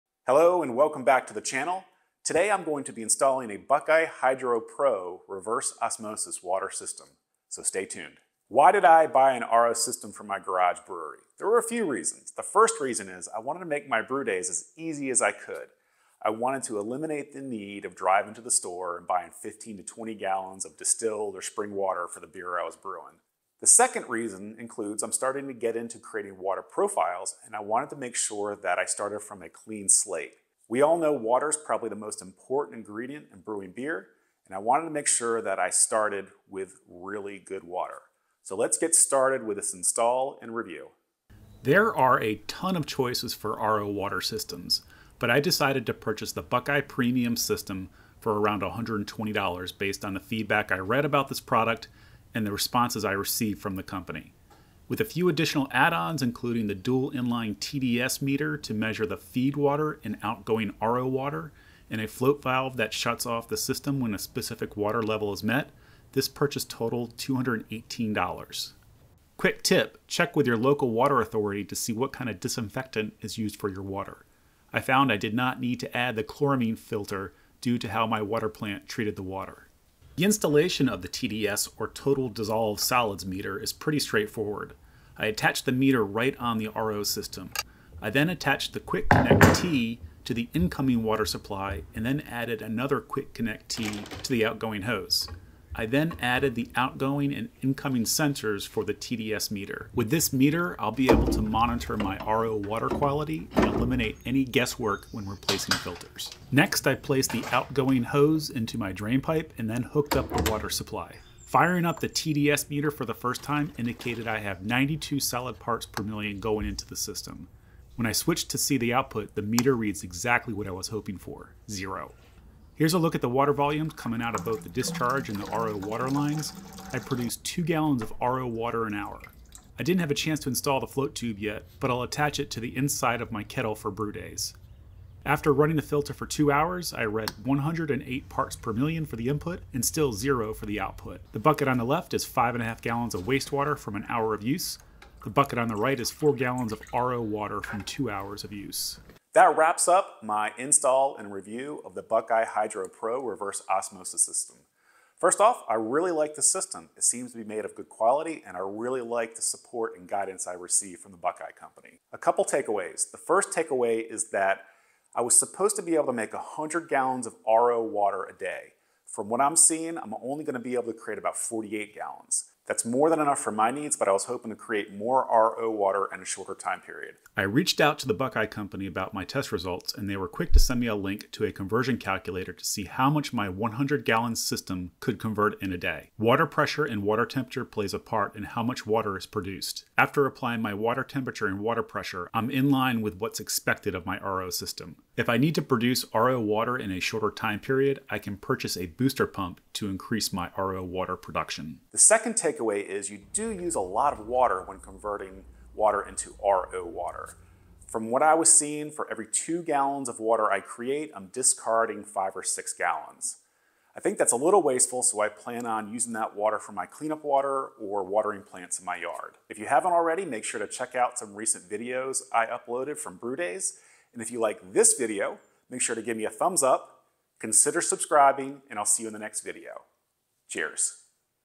How to Install Reverse Osmosis Water System for Homebrewery [ Buckeye Hydro Review ]
We all know water can greatly impact the quality of beer! See why I chose to purchase and install a Buckeye Hydro Pro Reverse Osmosis system in my garage home brewery, the add-ons I decided to included in the system, any my thoughts on how this system performed.